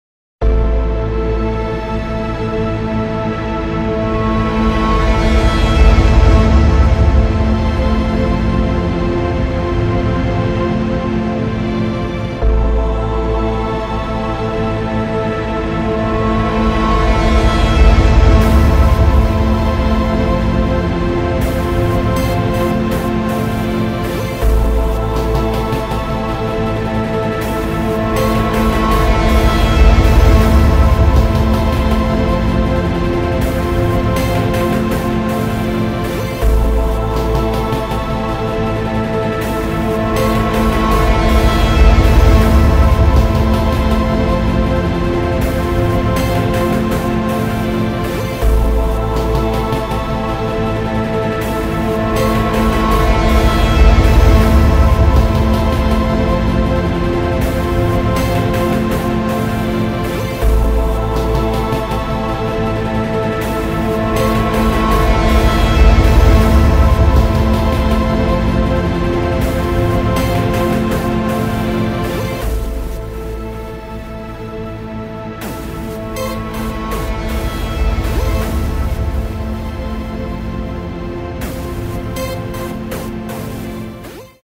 Sound Demo 3
And another one, more dramatic this one is quite tron inspired-ish and i'm quite satisfied on how this came out.